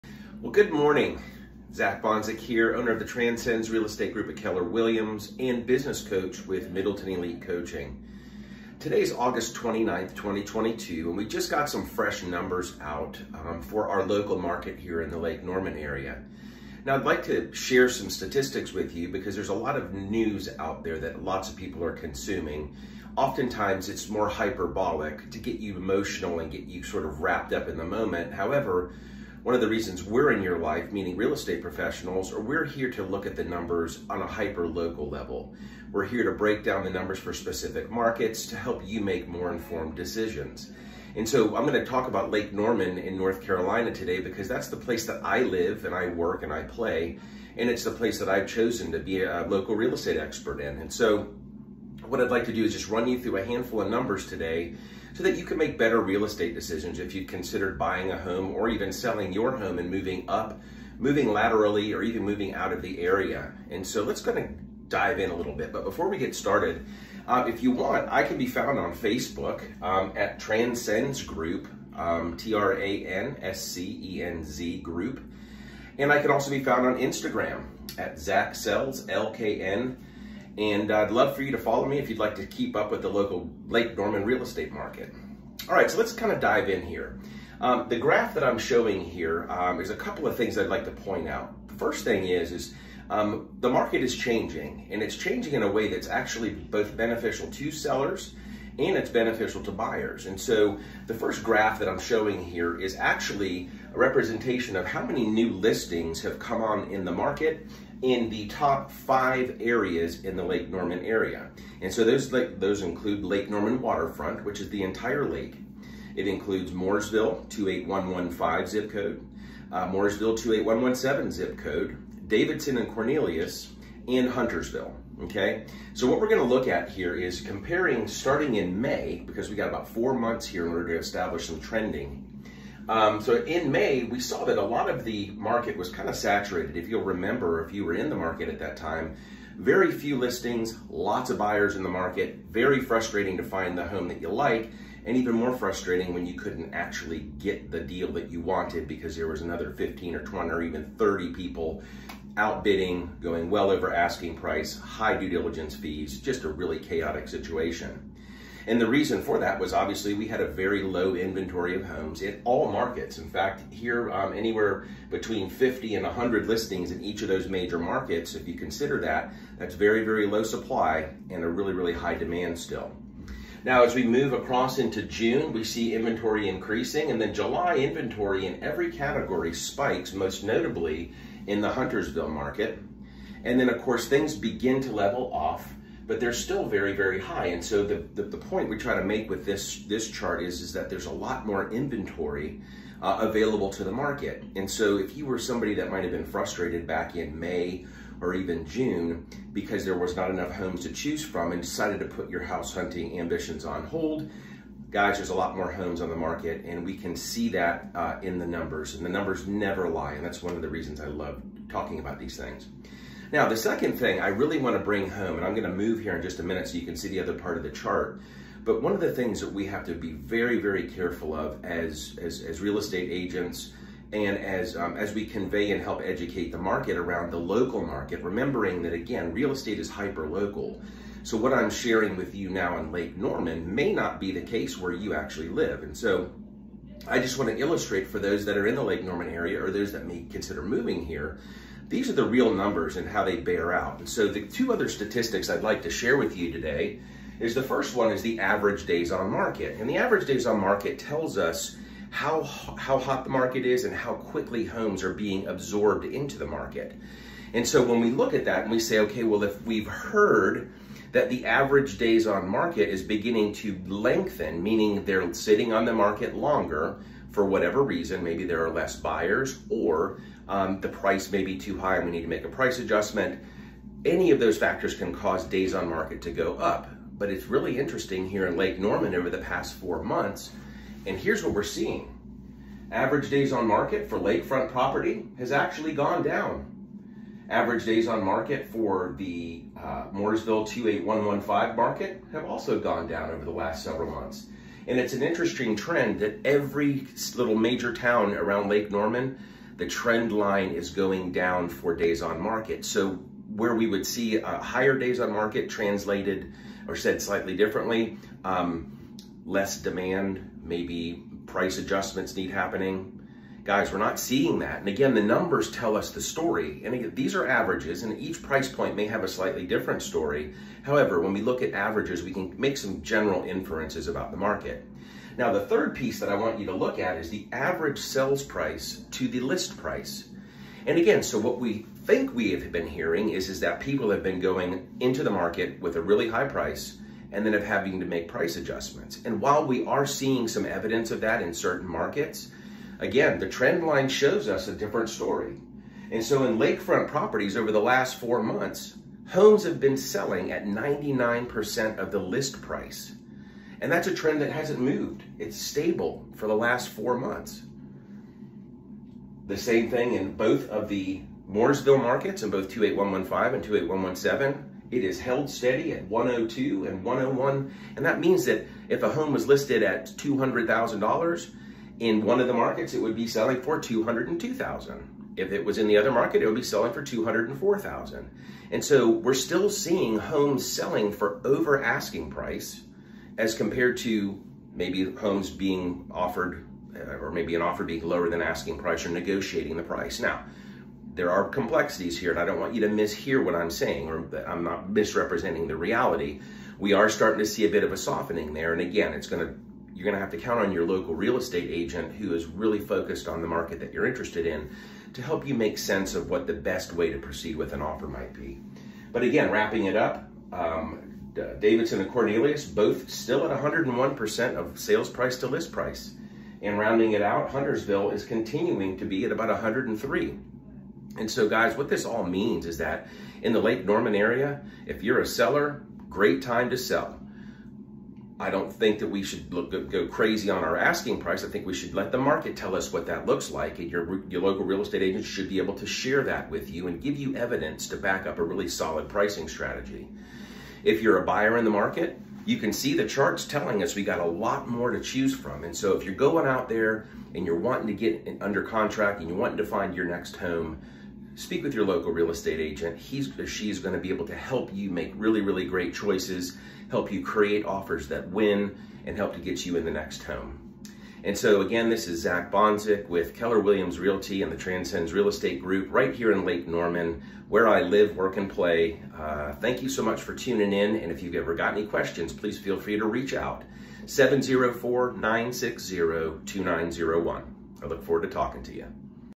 Lake Norman Real Estate Outlook August 2022
All real estate markets are hyper local and this is the latest Lake Norman real estate market assessment by Zack Bonczek, owner of The TRANSCENZ Real Estate Group at Keller Williams and business coach at Middleton Elite Coaching.   If you’re in the Greater Charlotte, North Carolina real estate market, let’s connect!